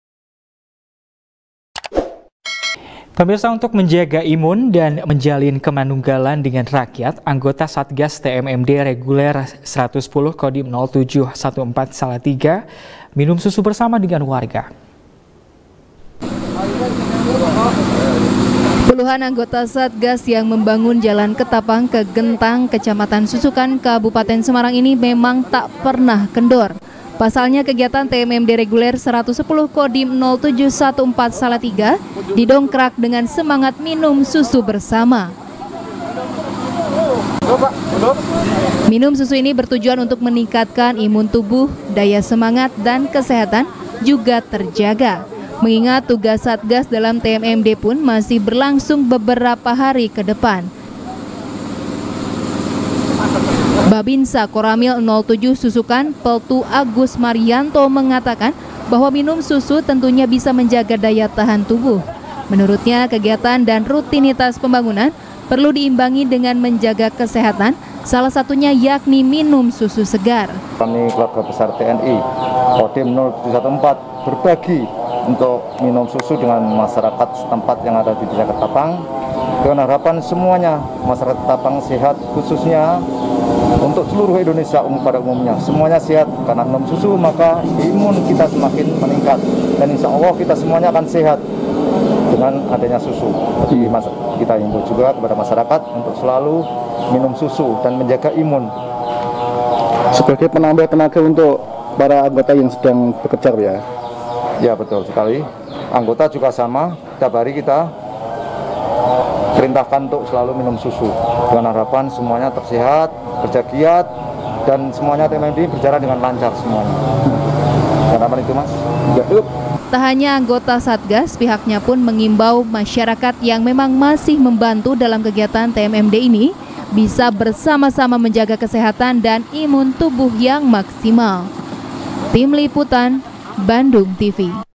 Puluhan Anggota TMMD Ini Minum Susu Bersama Warga | SBR | BANDUNGTV
SBR | BANDUNGTV | IRFAN RAMANDA | Kemanunggalan Dengan Rakyat, Anggota Satgas TMMD Reguler 110, Kodim 0714 Salatiga, Minum Susu Bersama Dengan Warga.